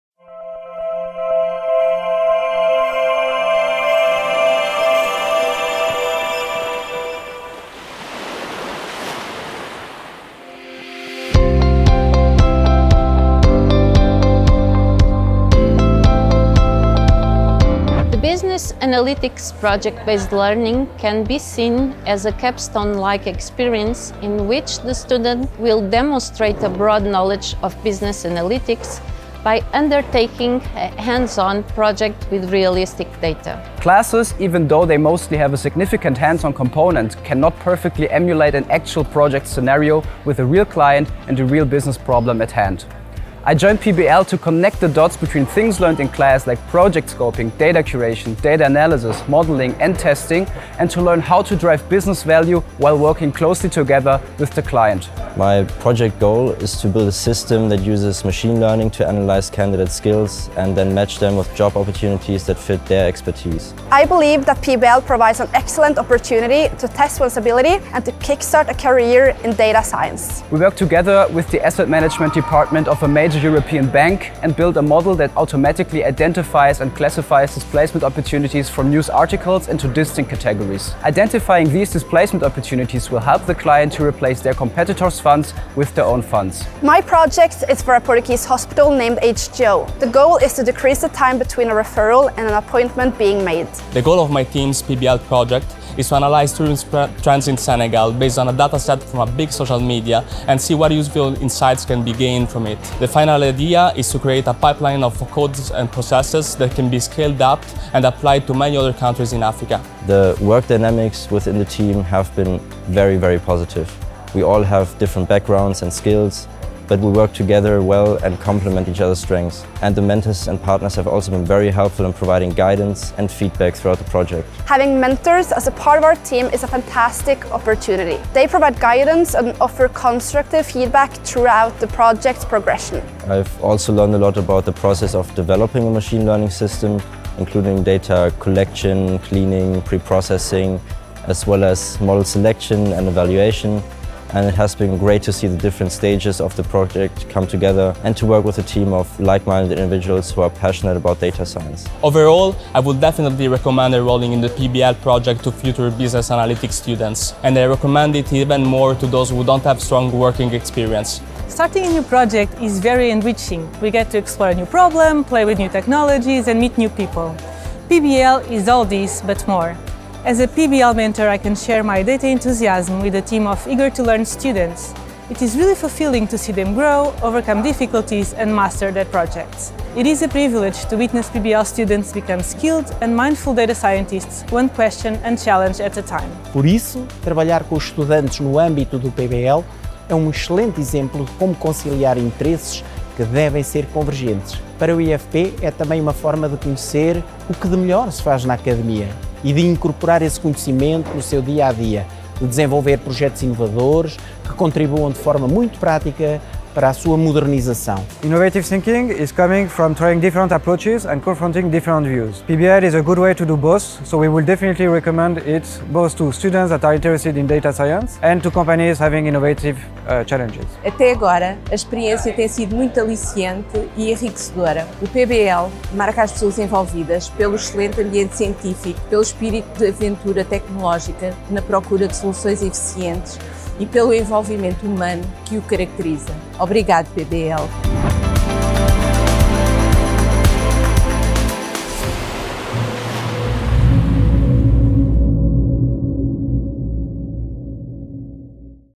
Master's in Business Analytics | Students, Partners & Mentors' experience on Project-Based Learning
Project-Based Learning (PBL) is a unique learning experience. It allows Master’s in Business Analytics students to work on a real-world data challenge from the very beginning of the program, supported by a team of professional mentors, where they can apply their knowledge and tools as they learn them in class.

Learn more about PBL with the students, partners, mentors who lived this experience in 2022/23!